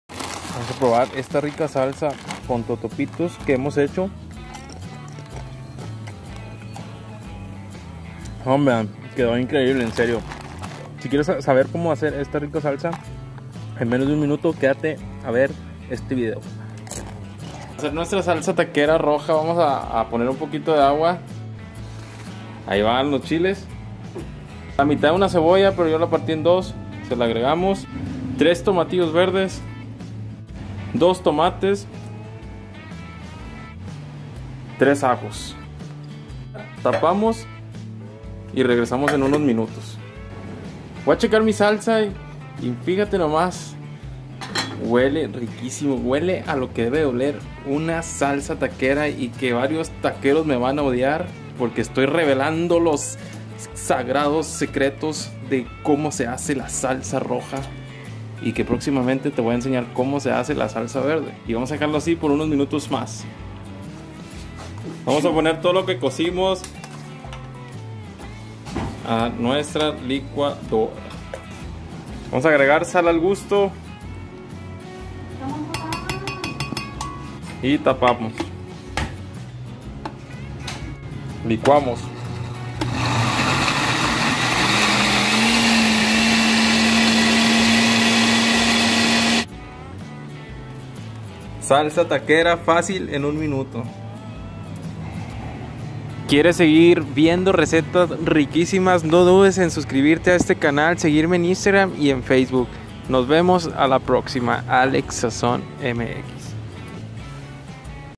El SECRETO de la salsa que los taqueros no quieren que conozcas || TE ENSEÑO COMO || AlexSazon Mx
Hola amigos! No te ha pasado que vas a unos tacos y te quedas con la duda de como hacen esa salsita tan deliciosa en este nuevo vídeo te muestro como hacer esa salsita que los taqueros usan para dar le mas sabor a sus tacos!